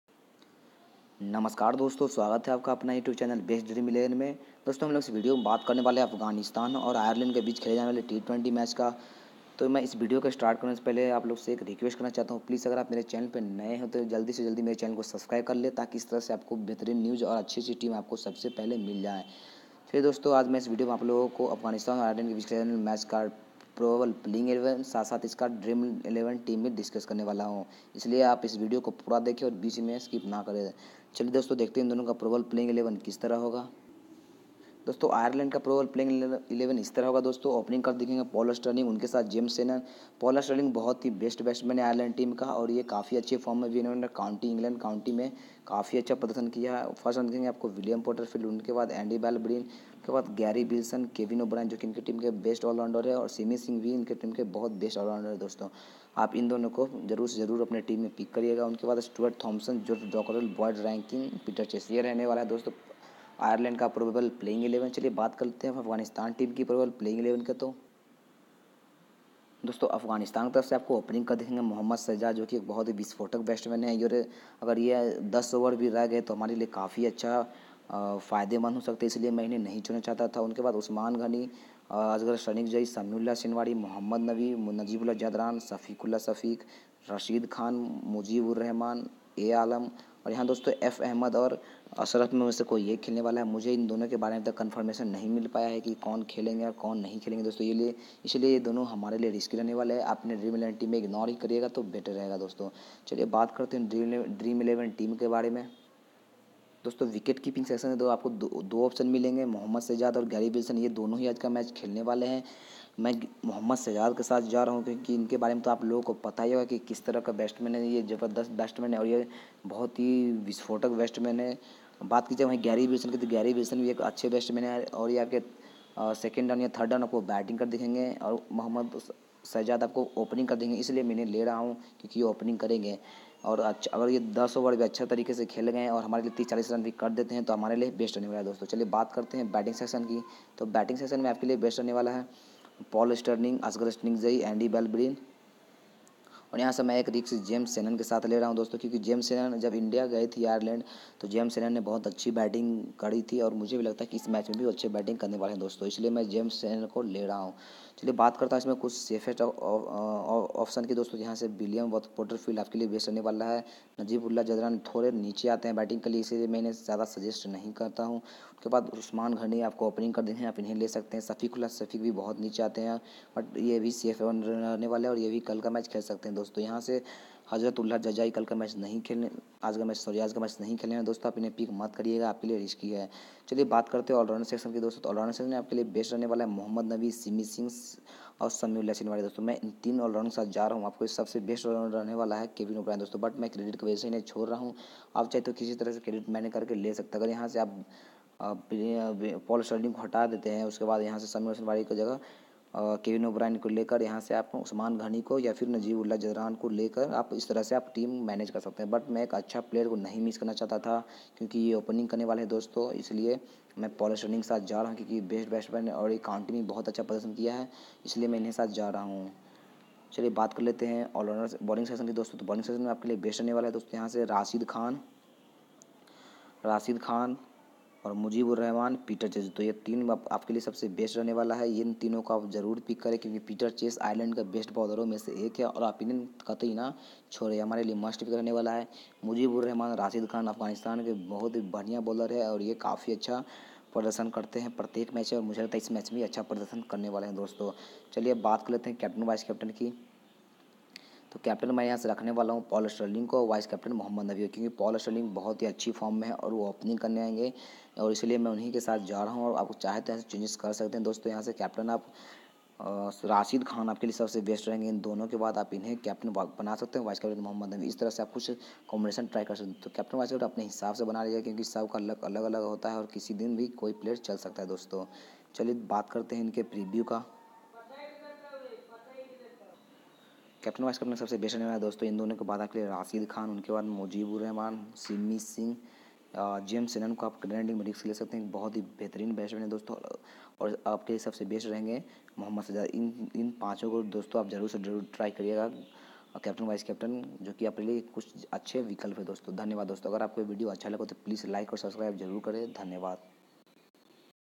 IRELAND VS AFGHANISTAN PROBABLE PLAYING11 AND DREAM11 TEAM
Hii friends aaj mia apko ire vs afg ke bich khelejane wala T20 match ka possible playing11 and dream11 team de rha Hu agr apkko mera video Accha lge to please like and subscribe jarur kare thank y